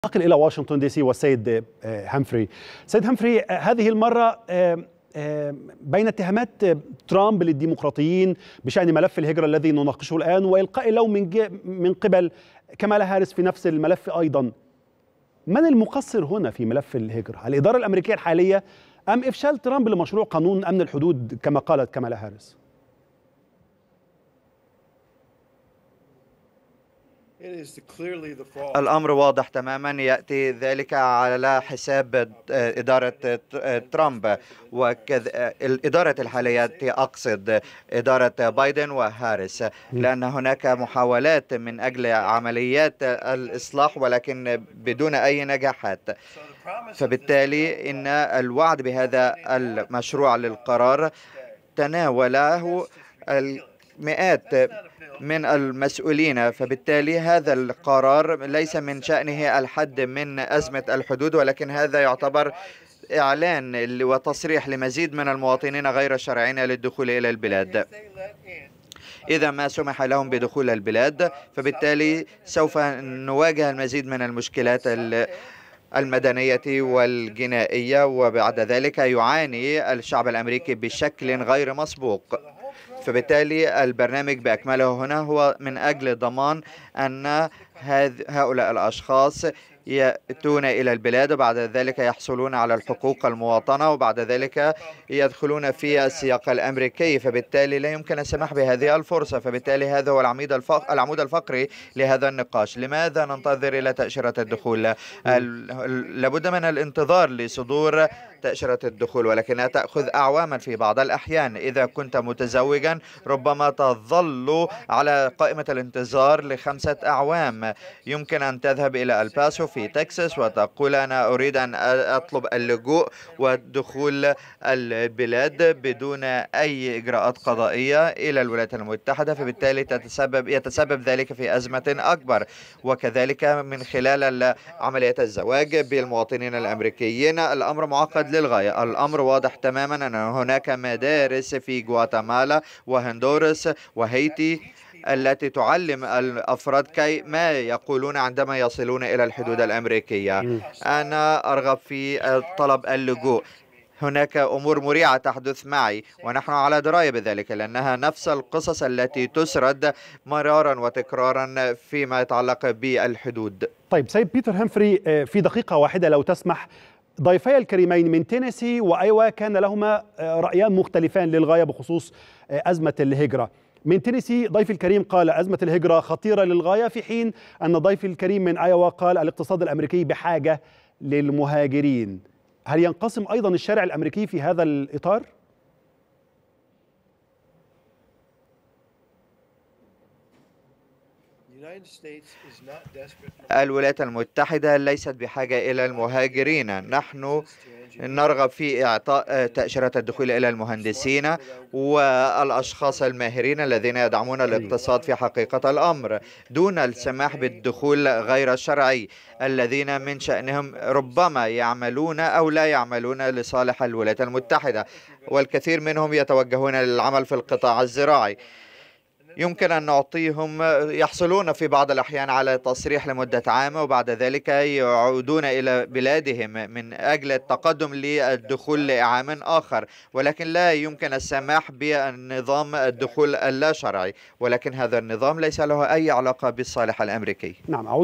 بيتر همفري دبلوماسي أمريكي سابق: الولايات المتحدة ليست بحاجة إلى المهاجرين
11/9/2024